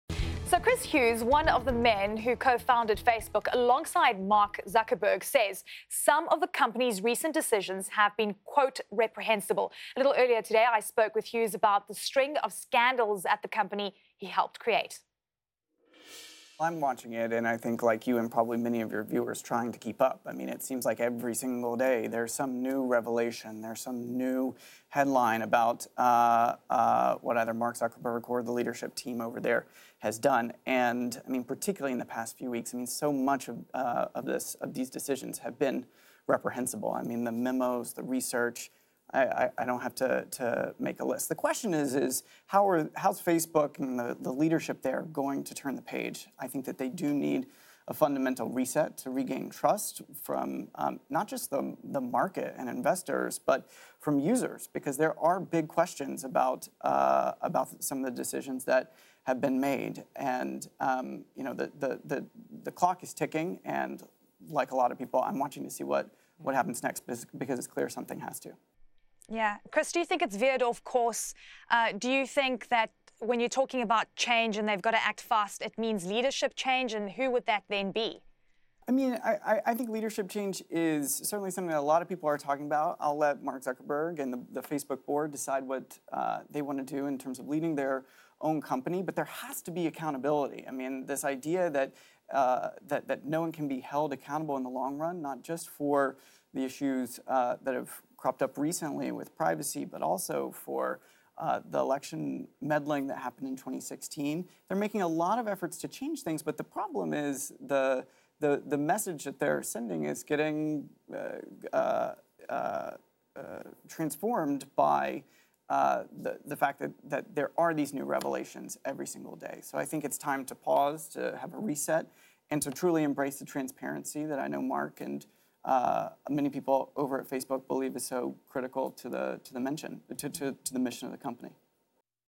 Facebook founder: Social Network's decisions 'reprehensible'
Chris Hughes, co-founder of Facebook, says "there has to be accountability" in regards to recent scandals at the social network. He believes leadership needs a "fundamental reset to regain trust" from both the market and users.